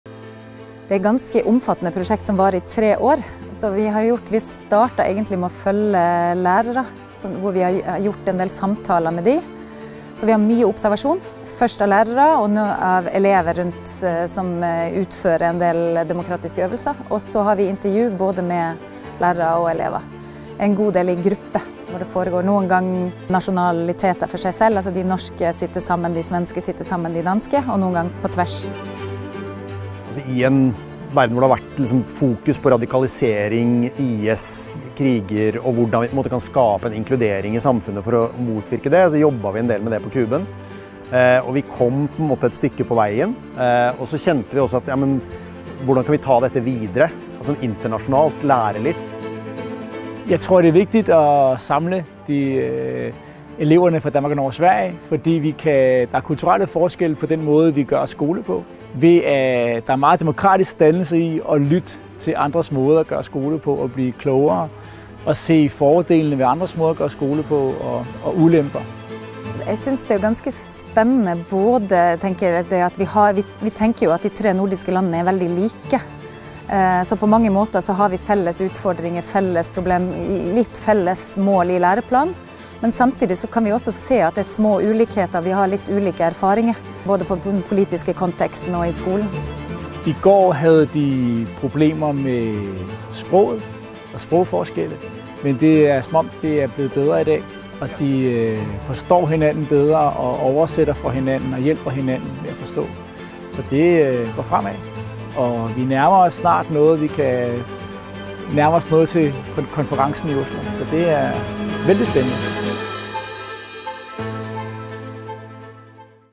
Norsken, svensken og dansken og demokratiopplæring.
Bli med til den danske byen Tisvilde for å se hvordan elever fra Kuben videregående skole, Bjurgården gymnas, Fredriksberg gymnas, deres lærere og forskere fra Universitetet i Oslo jobber med demokrati og utenforskap.  (video: Shane Colvin & Magnus Heie/UiO).